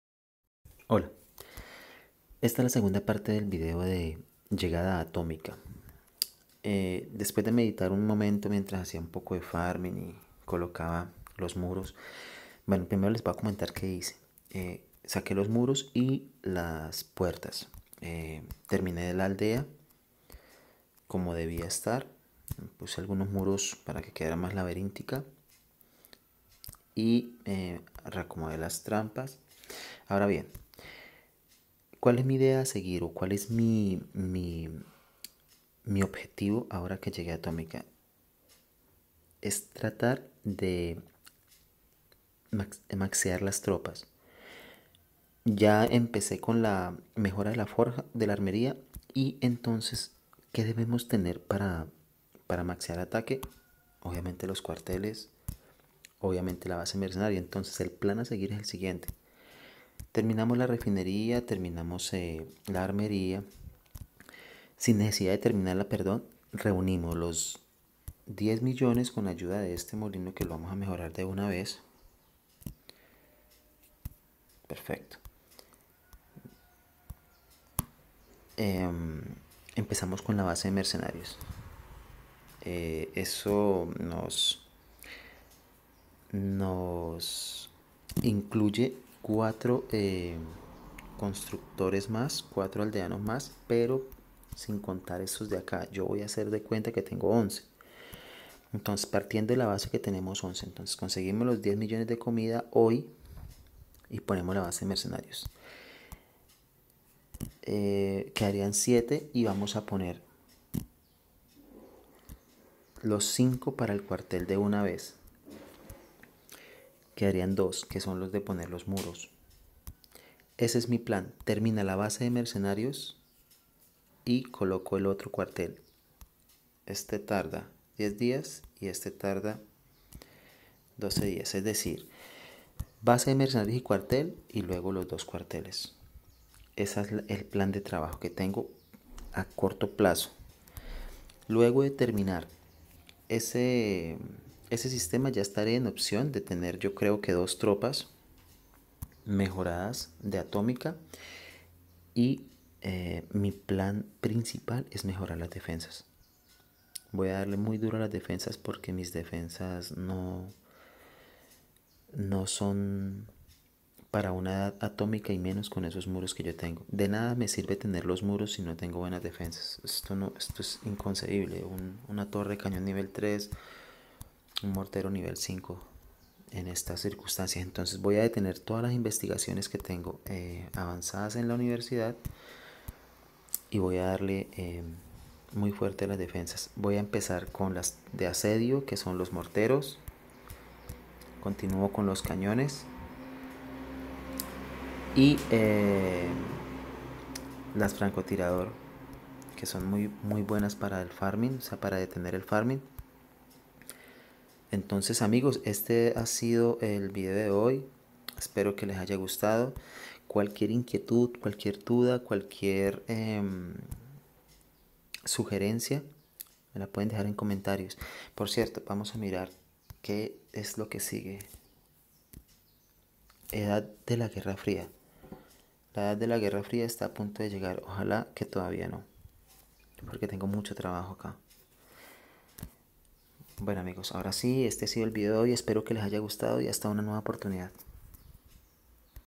Llegada de la Edad Atómica II - 05.03.2017
Segunda parte de las primeras impresiones y plan de mejoras a mi aldea, olvide mencionar que para mejorar la biblioteca debe estar sin investigación activa. Enjoy